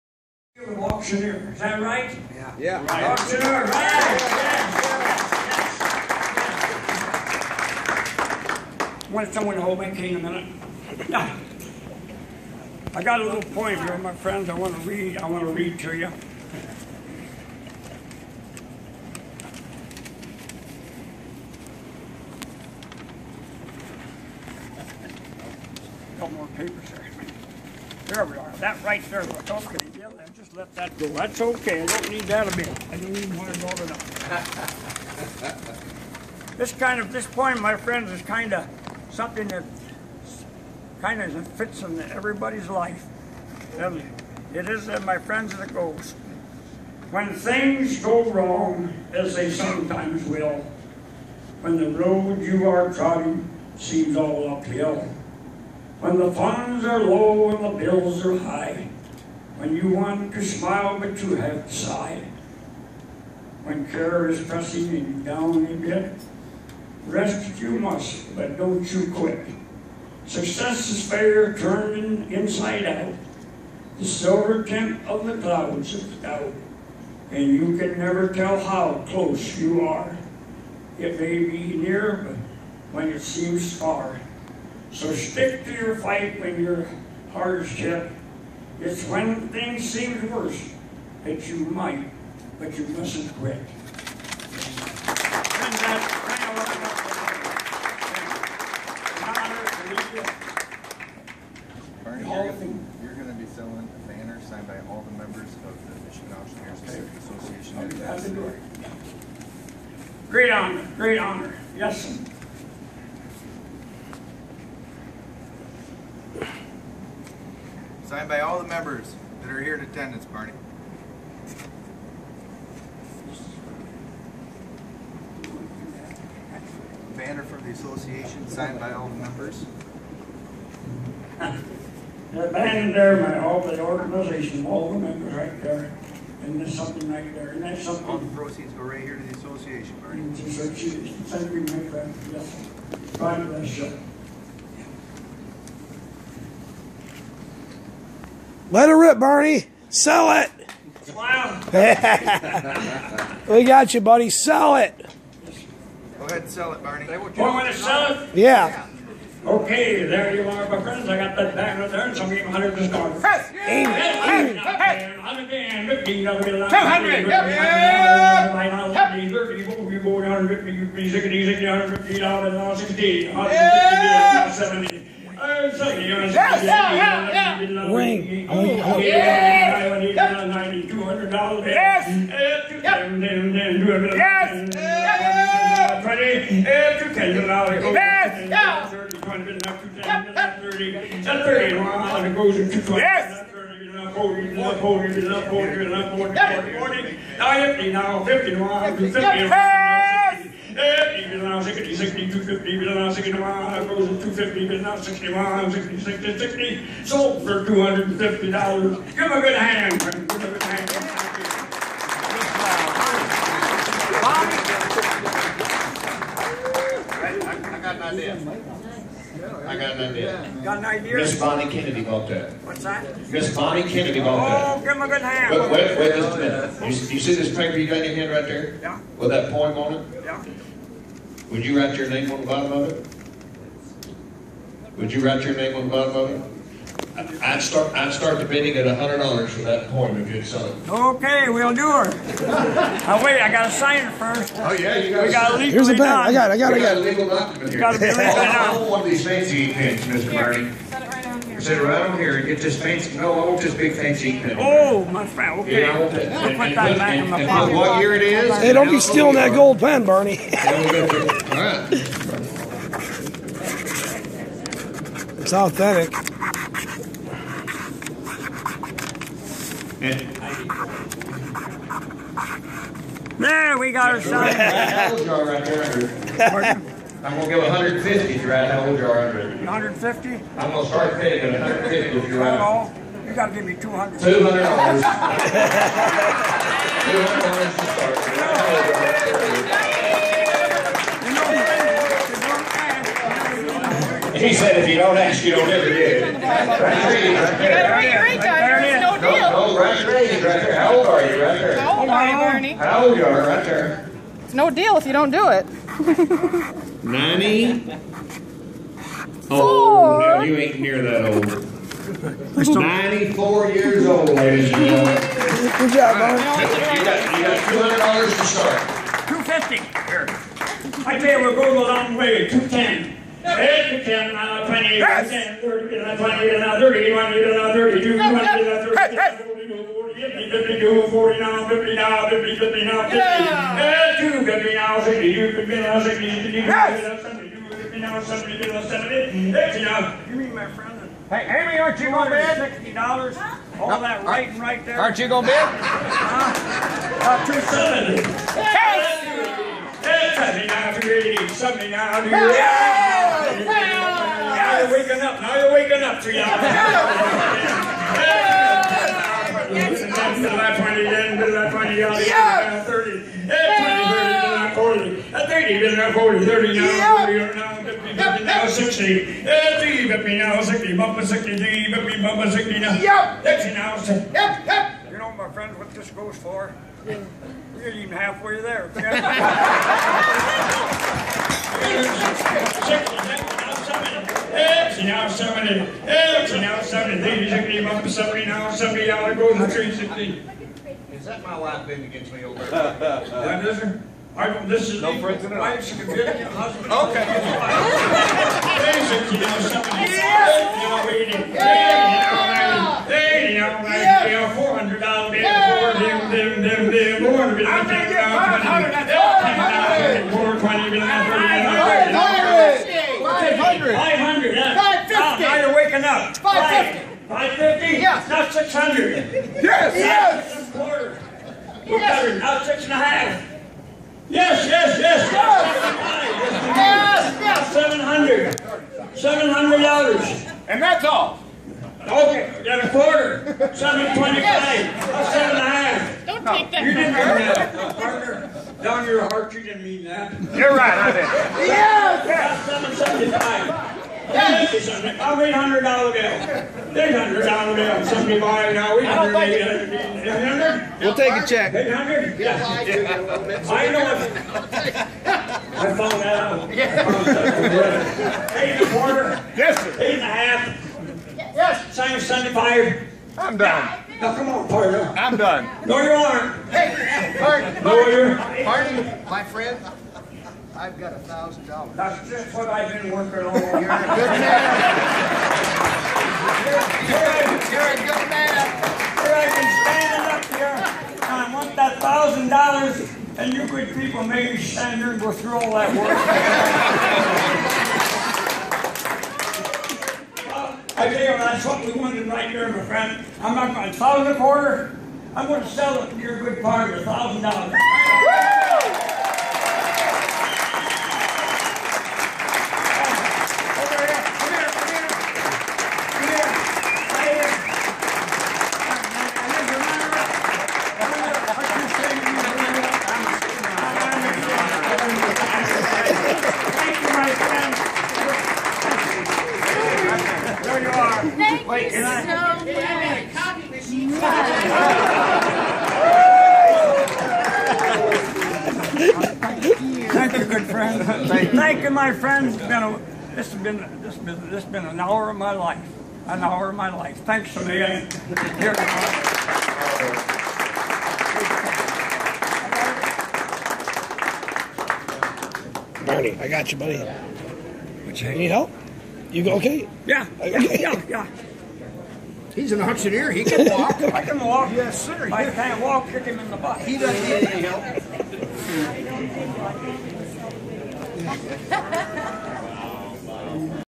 94 Year Old Auctioneer, Barney Barnhart
94 Year Old Barney Barnhart Brings the House Down while Spanky & Amy Assiter bid generously at Michigan Auctioneers Association Benefit Auction.